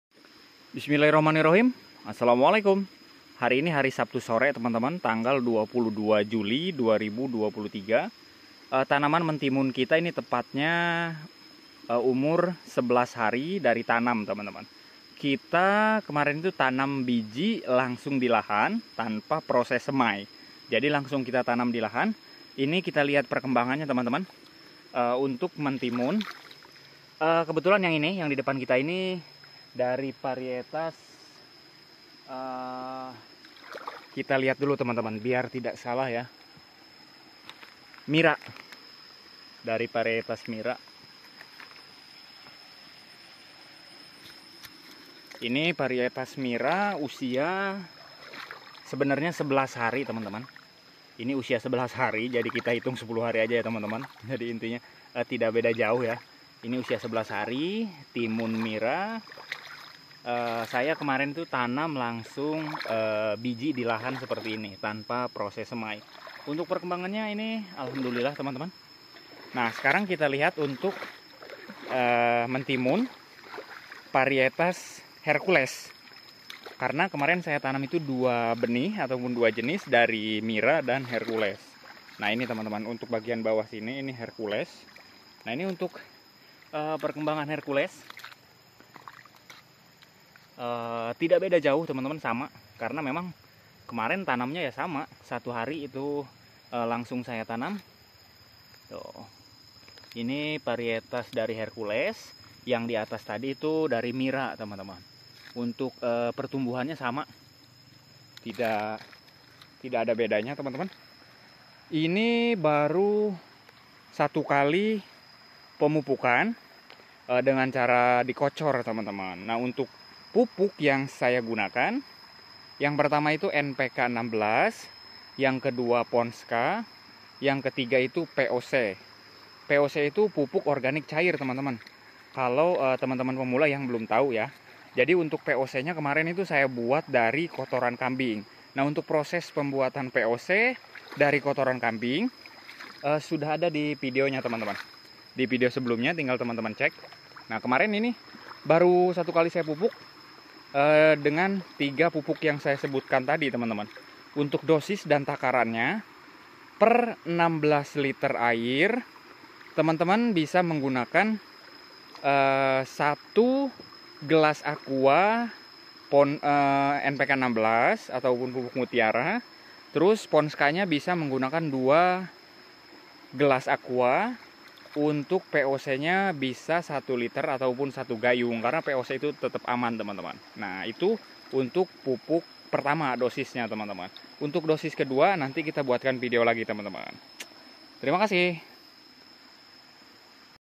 timun usia 10 hari setelah tanam
tanaman mentimun usia 10 hari setelah tanam di lahan tanpa proses semai